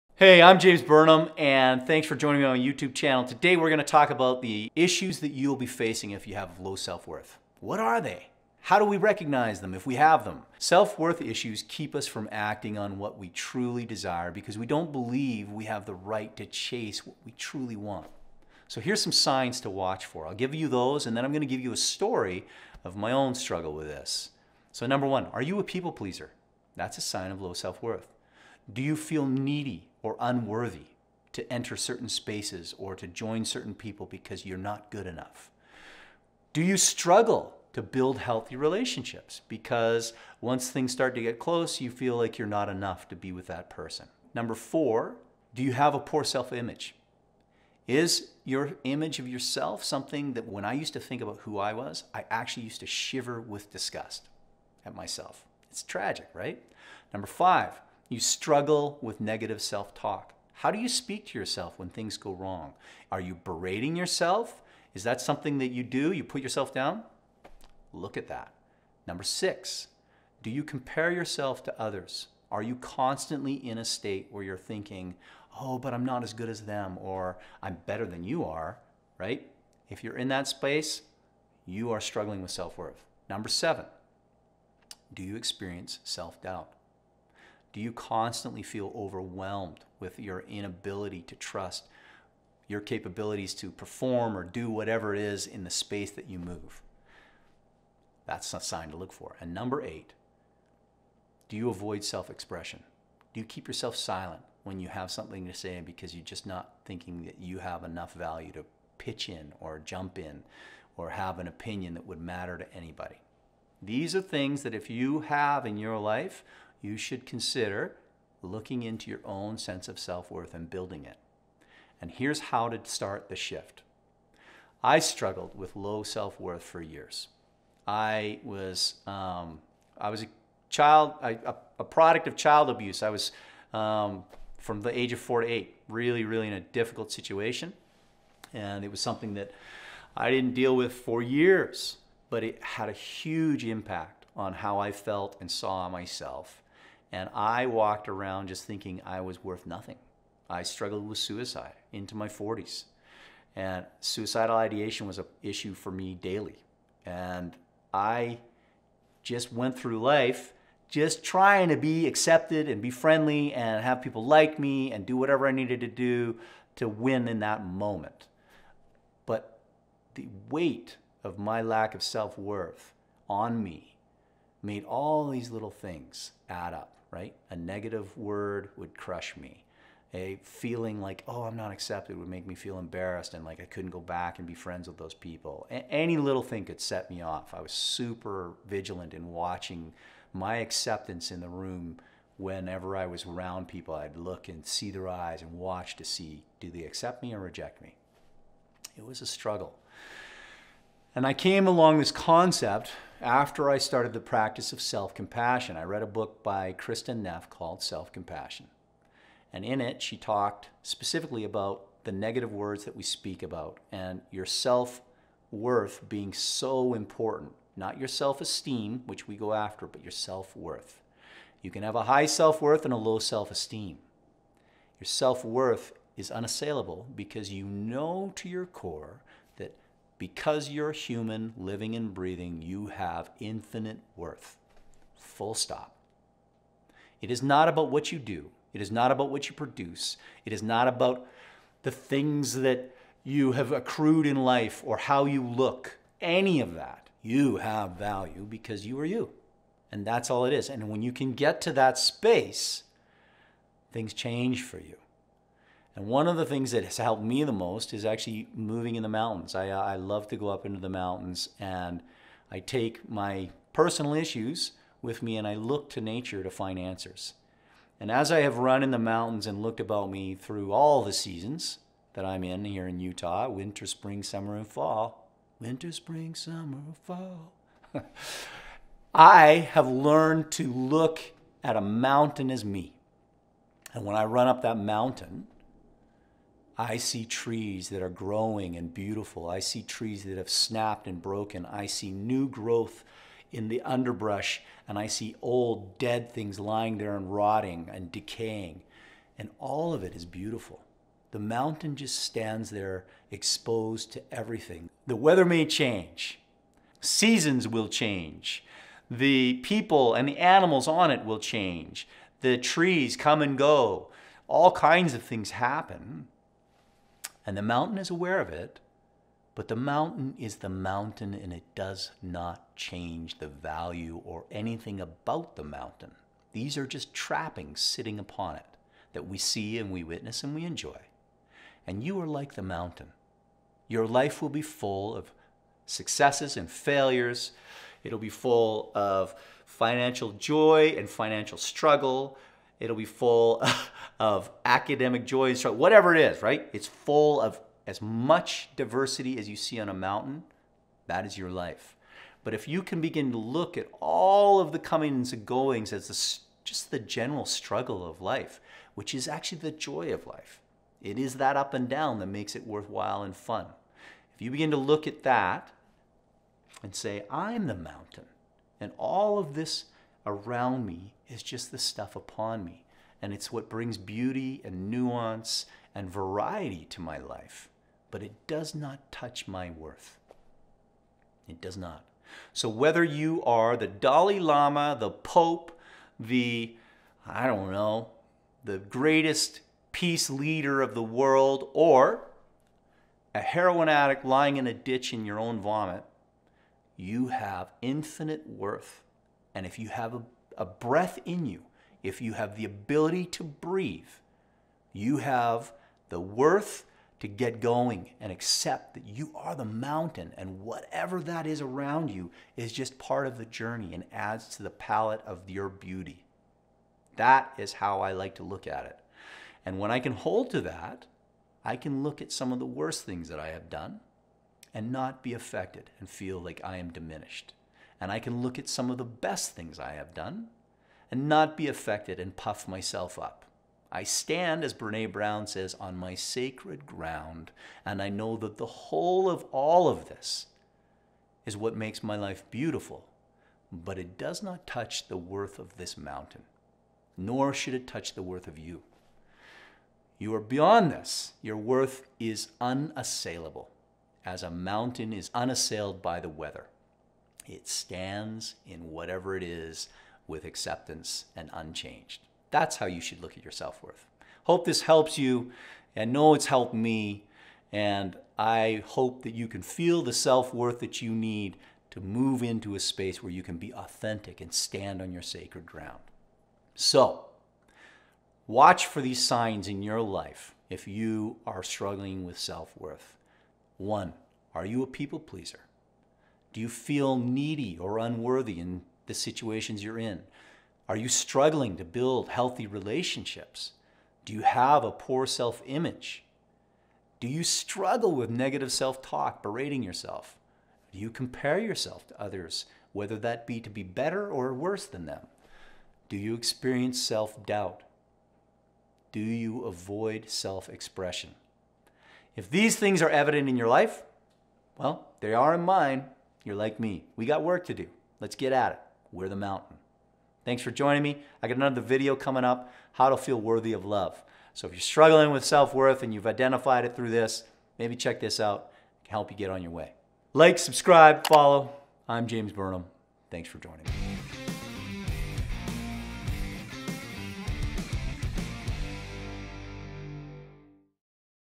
What Are Self Worth Issues | Finding Your Identity with James Burnham
Do you know what self worth issues are?  How can you recognize self worth issues in your own life?  Watch this video to learn what are self worth issues and how you can overcome them.

Welcome!
I'm James, speaker, coach, and expert at building excitement and creative energy in the face of the fear and uncertainty.

00:00 What are self worth issues?
00:26 Signs of low self worth
02:10 My story
03:25 Self worth
04:31 Your life is a mountain
06:38 You have infinite worth